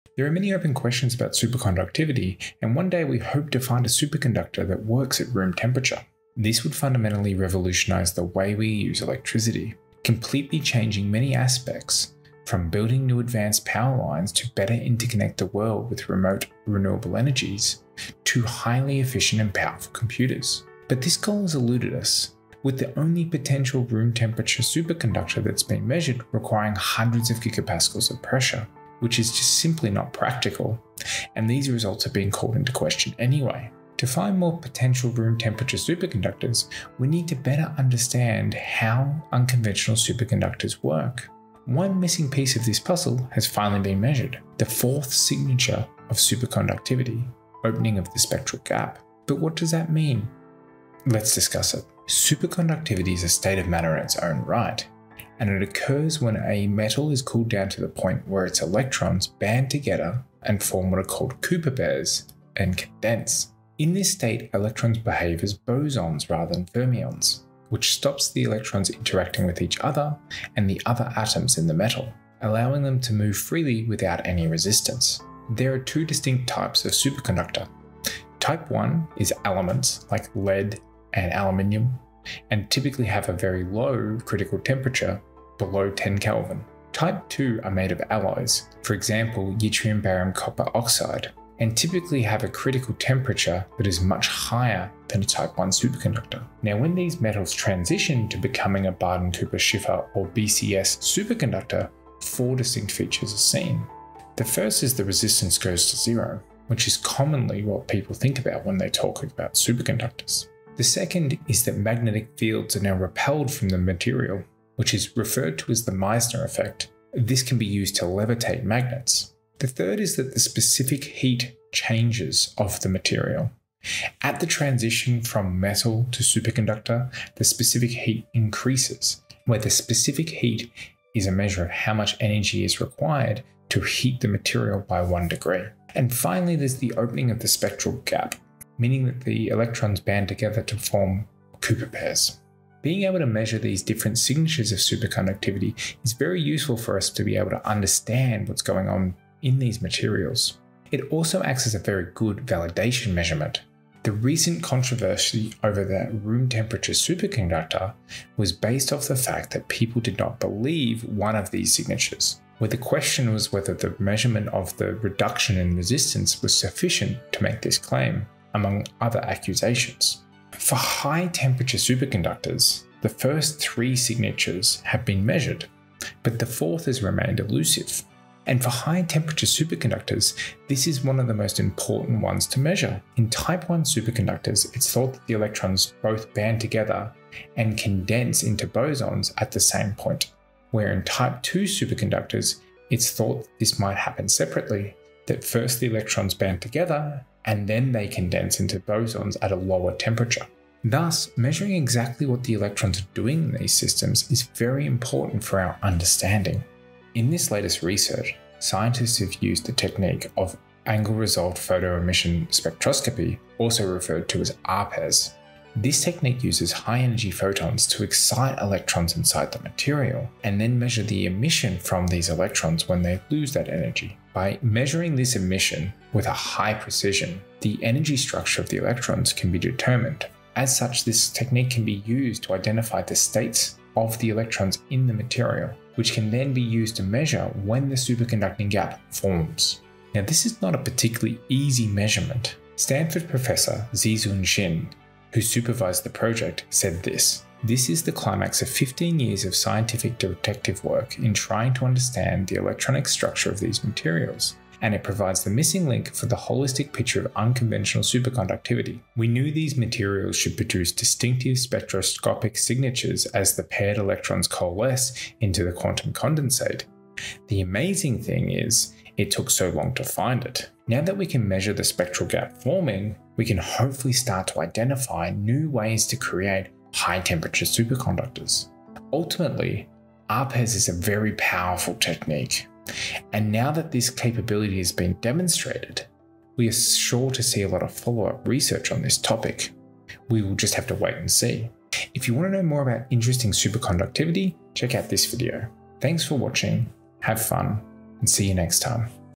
4th Superconducting Signature Observed
There are many open questions about how superconductivity forms. Ultimately, we want to understand this so that we can make a room-temperature superconductor. In a Bardeen–Cooper–Schrieffer (BCS) superconductor 4 distinct changes occur at the transitions: the material has zero resistance, it now exhibits the Meisner effect, the specific heat increases, and the electrons band together to form cooper pairs. In high-temperature unconventional superconductors, this banding together of electrons is not well understood. As such, it is useful to try and image this process to better understand it. In this latest research [1, 2], scientists have used the technique of angle-resolved photoemission spectroscopy referred to as ARPES to image this transition from normal fermionic electrons to bosonic cooper pairs. This is a proof of principle experiment that holds a lot of promise for investigating some of the high-temperature and high-pressure superconductors and may be useful in clearing up the controversy surrounding the recently measured room-temperature superconductor [3, 4]

0:00 - Intro
0:51 - Superconductors
3:25 - Latest measurement (ARPES)